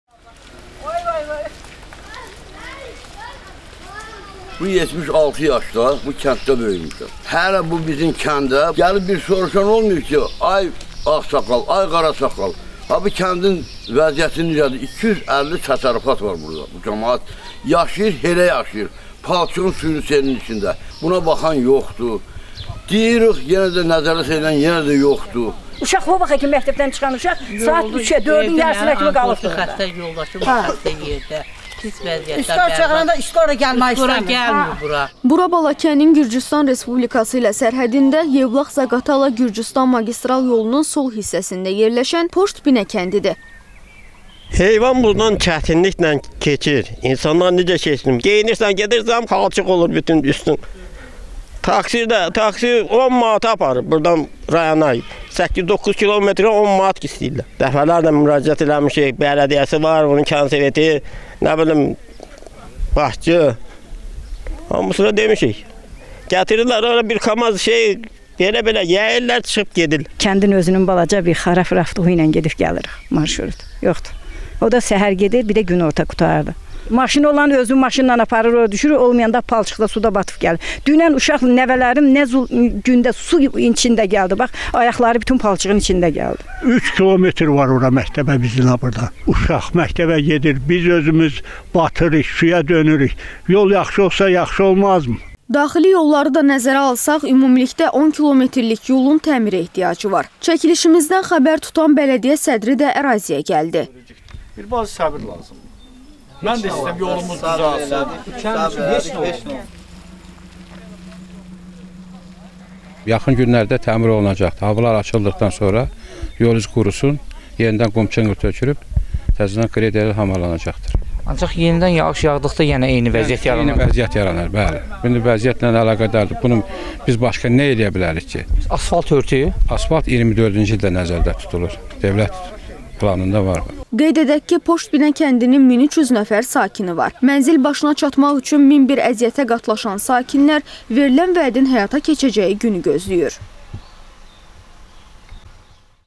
Balakəndə keçilməz kənd yollarının aqibəti necə olacaq?
Balakənin Gürcüstanla sərhədində yerləşən Yevlax-Zaqatala-Gürcüstan magistral yolunun sol hissəsində yerləşən Poştbinə kəndinin sakinləri yolun yararsız vəziyyətdə olmasından şikayət edirlər.
Kənd sakinlər deyir ki, problemlə bağlı aidiyyəti qurumlara dəfələrlə müraciət etsələr də, yol təmir olunmur.
“Camaat palçığın, suyun içində yaşayır”, – deyə yolda hərəkətin çətinliyindən danışan kənd sakini qeyd edib.
Sakinlər rayon mərkəzinə getməyin də onlara baha başa gəldiyini vurğulayıb: "Taksilər rayonun mərkəzinə 10 manata gedirlər".